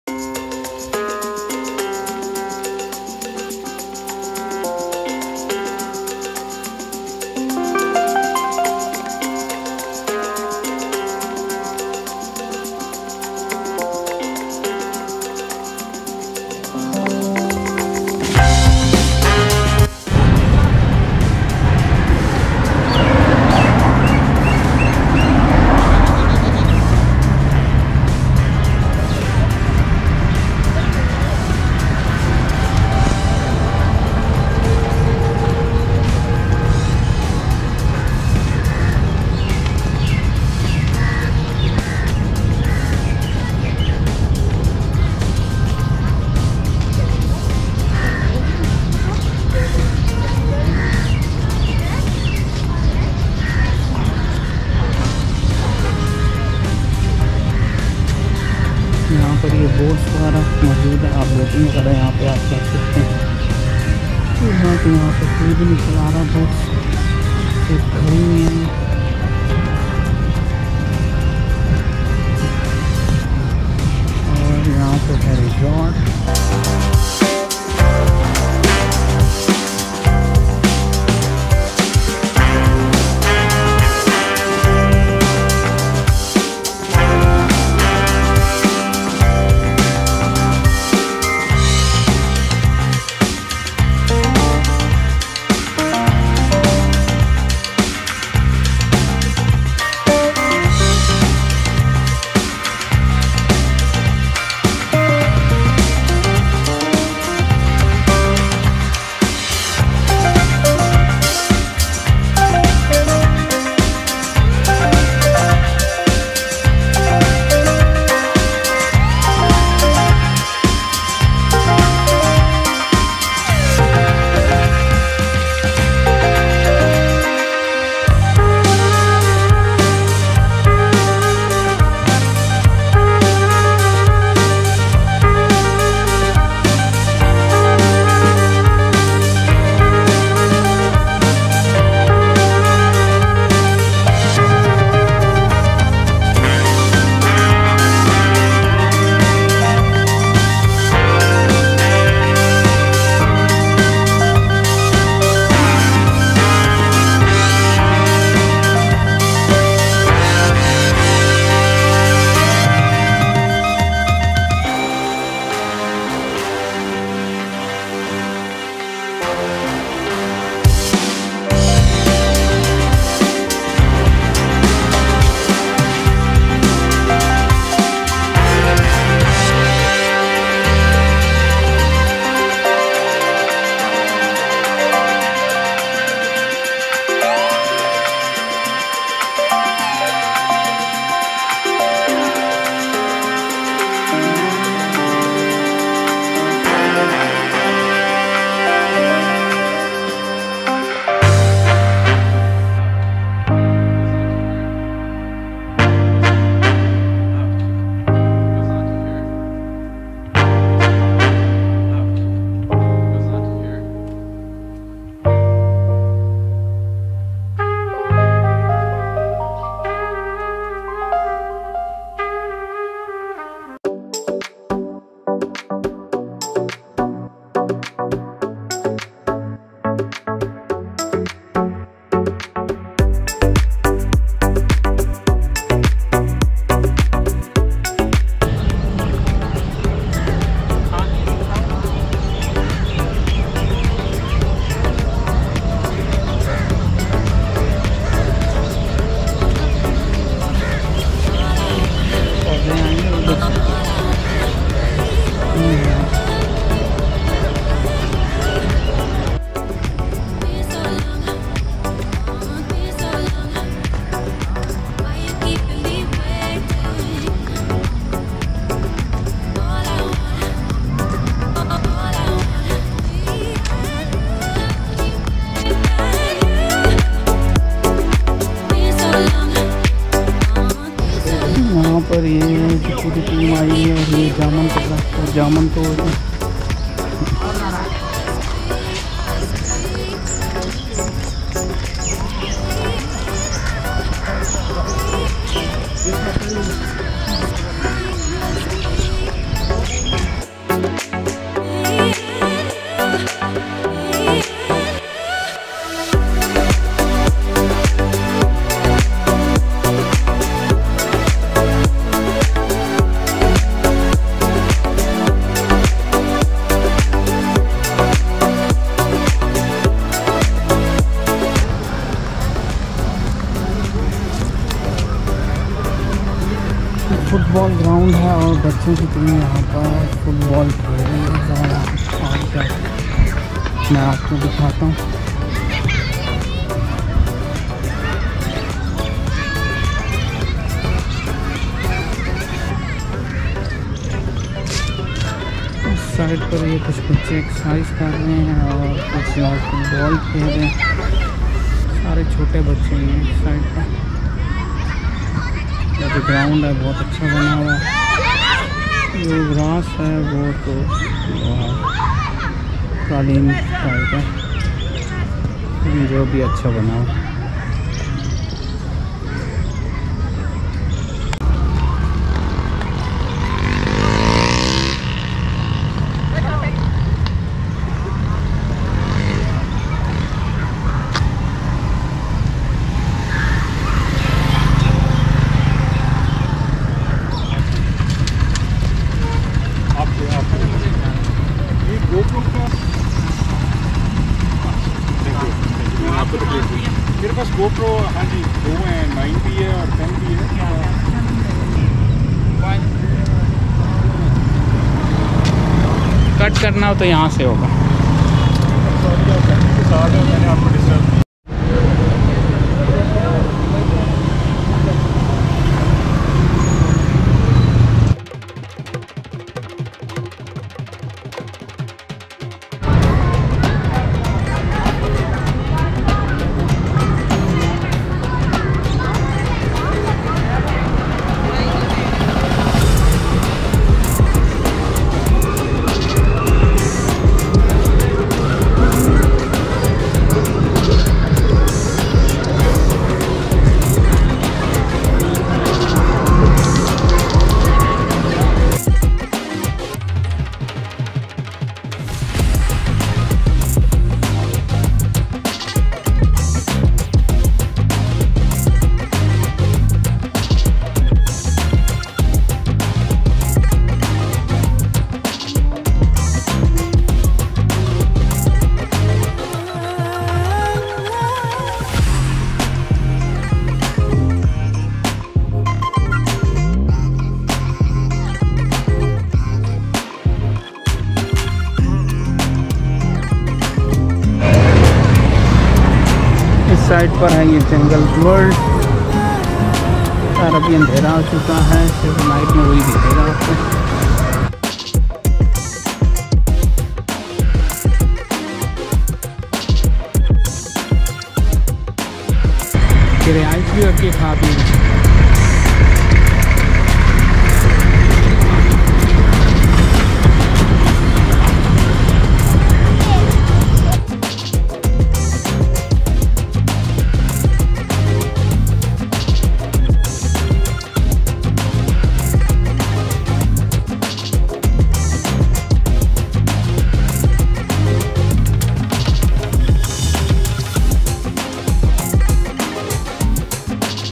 Walking in the Rawalpindi Pakistan | Ayub National Park | HD | Virtual Walk With Us
Ayub Park Rawalpindi | Jungle World Zoo | Bhoot Bangla Ayub Park | ایوب پارک